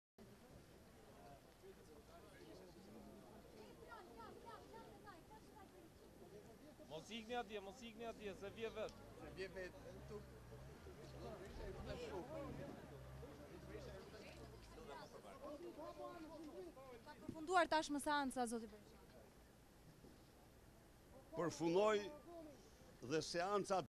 A1 Report - VIDEOLAJM/Berisha jo dhe aq i nxehur, kujdeset për flokët
TIRANE - Ndonëse seanca plenare e kësaj të enjteje u përfshi nga debate të ashpra, ish-kryeministri Sali Berisha nuk harroi të kujdesej për imazhin e tij para kamerave. Siç duket dhe nga kjo video e filmuar nga operatorët e A1 Report, zoti Sali Berisha teksa dilte nga salla e seancave plenare ka nxjerrë nga xhepi i xhaketës krehrin dhe ka nisur të krihet në publik. Më pas me qetësi i është afruar kamerave dhe ka nisur të komentojë atë çka ndodhi në aolën e Kuvendit.

Prej vitesh një tipar i veçantë i Berishës ka qenë dëshira e tij për të dalë në media por qëkur u larguar nga posti i liderit të Partisë Demokratik duket sikur kjo dëshirë është shtuar dhe më shumë. Madje gazetarët ndonëse janë gjithmonë të interesuar në opinionin e tij së fundmi siç dhe dëgjohet në video pa u shqetësuar presin që ish-kryeministri të afrohet vet para kamerave.